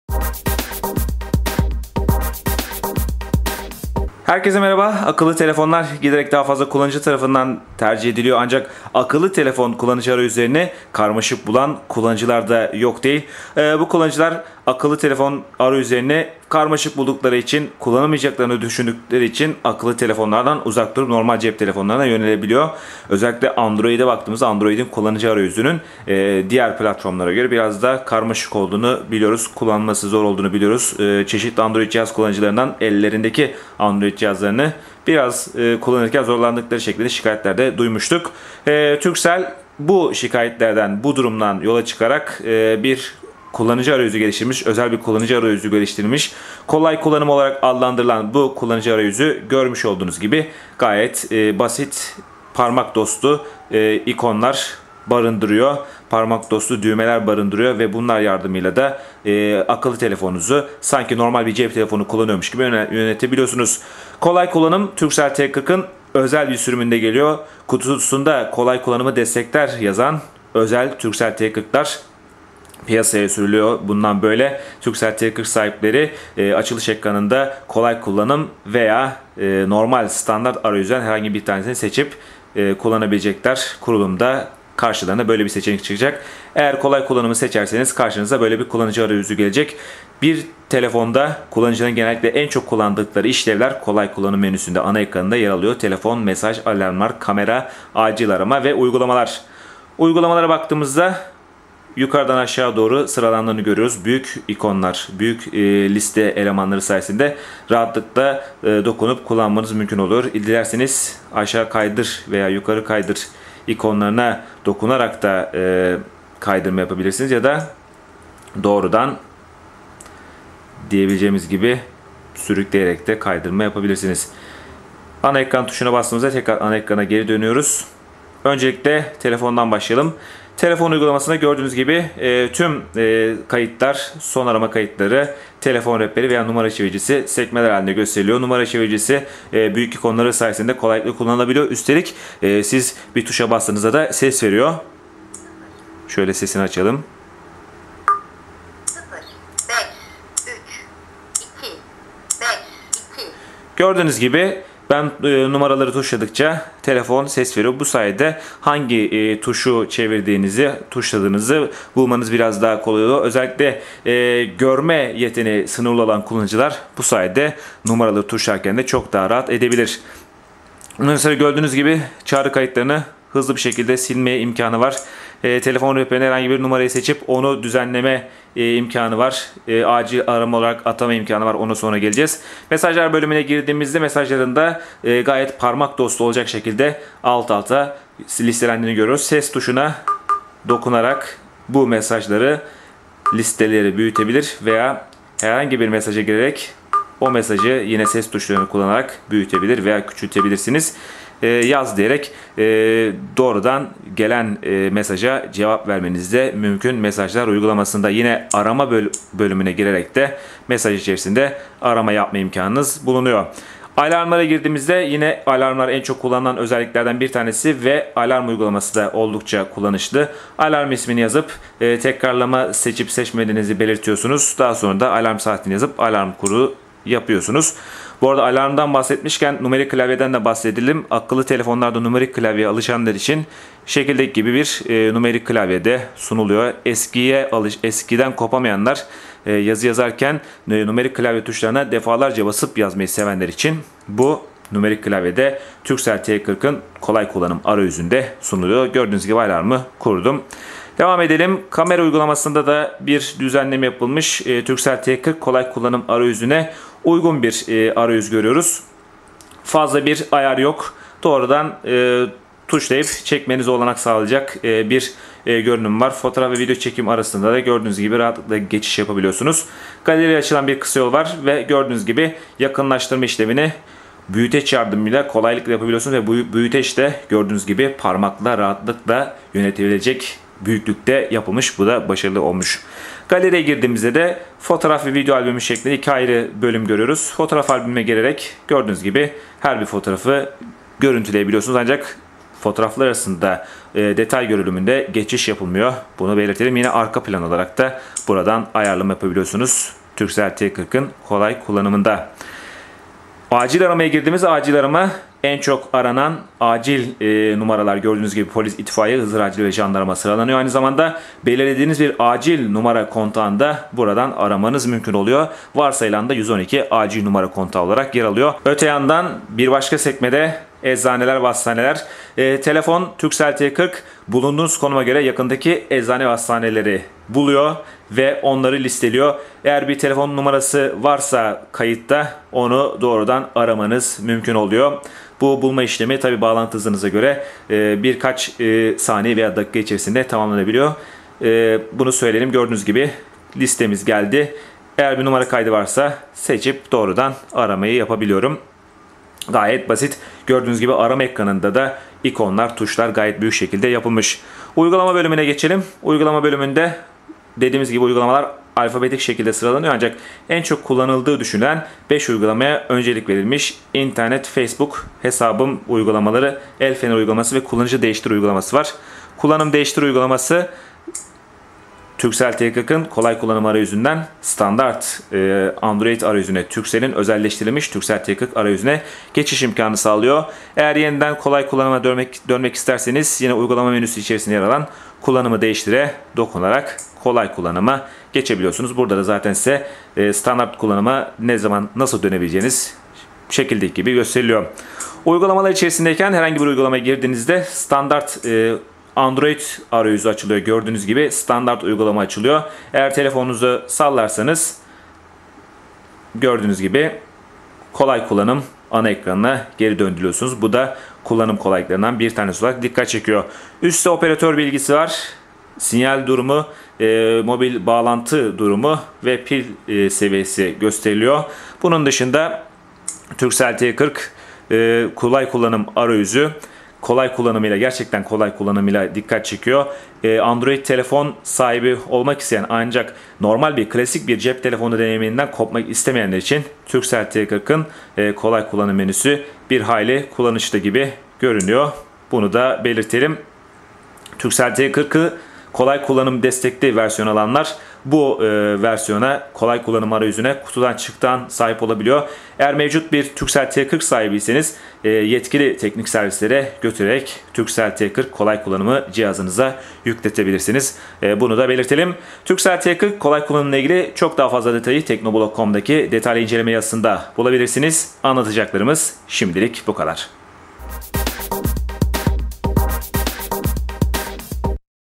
Turkcell T40 Kolay Kullanım İncelemesi
Turkcell'in kolay kullanışlı kullanıcı arayüzünü barındıran özel üretim Turkcell T40'ının detaylarına bakıyoruz.